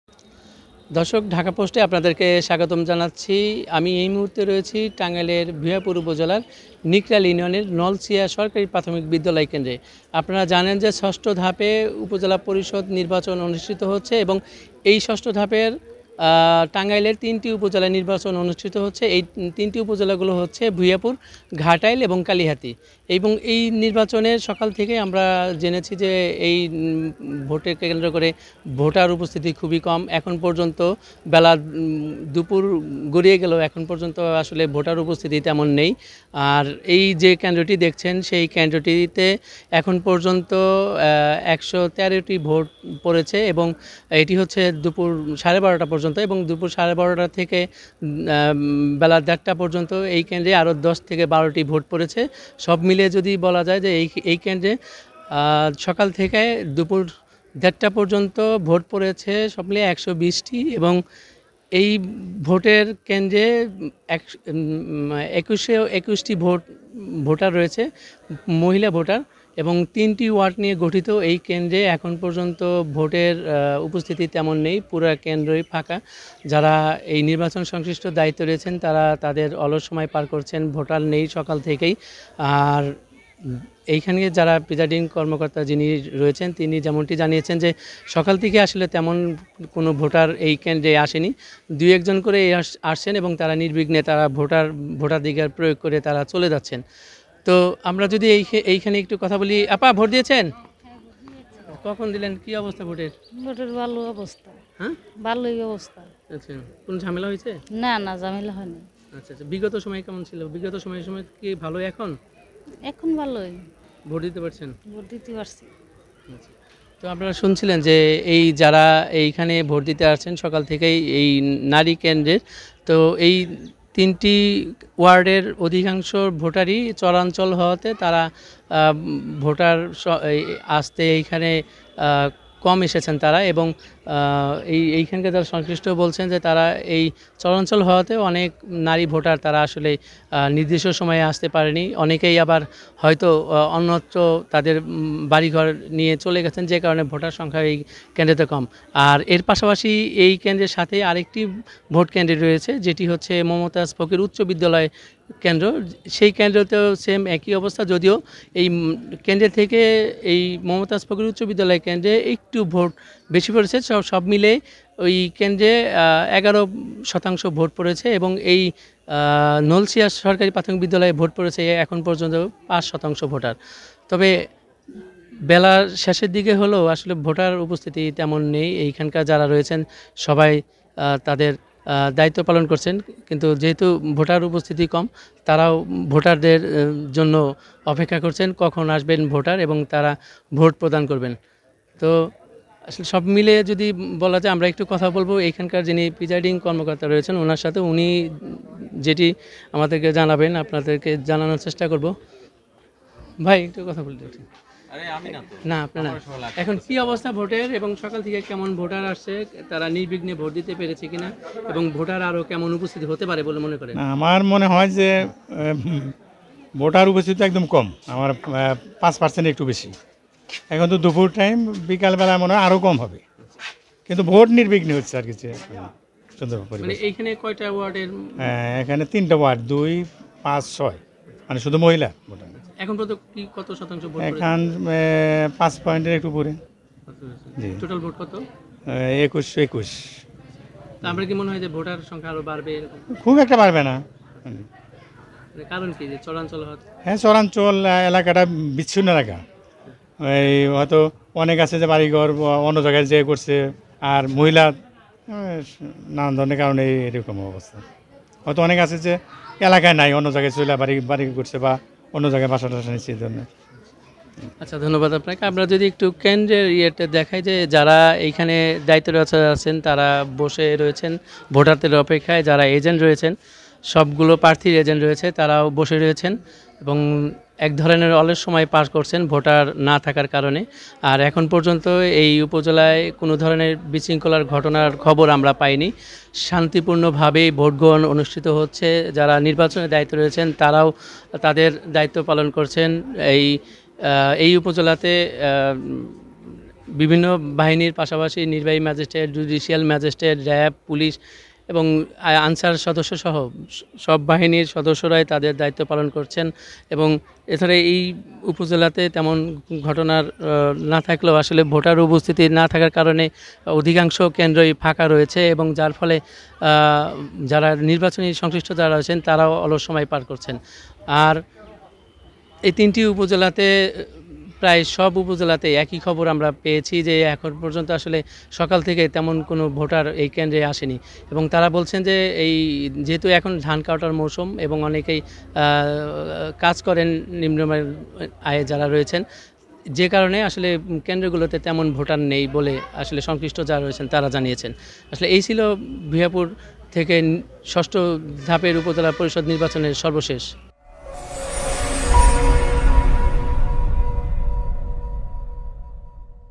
৬ষ্ঠ উপ‌জেলা প‌রিষদ নির্বাচ‌ন, দ্বিতীয় ধা‌পে তিন উপ‌জেলায় ভোট গ্রহণ | Tangail | Dhaka Post
উপ‌জেলার নল‌ছিয়া সরকা‌রি প্রাথ‌মিক বিদ‌্যালয় কে‌ন্দ্রে ভোট দুপুর দেড়টা ৫শতাংশ ভোট প‌ড়ে‌ছে। ফ‌লে ২১২১ ভো‌টের ম‌ধ্যে ভোট প‌ড়ে‌ছে ১২৩‌ভোট। ফ‌লে অলস সময় পাড়‌ কর‌ছেন নির্বাচনী সং‌শ্লিষ্টরা...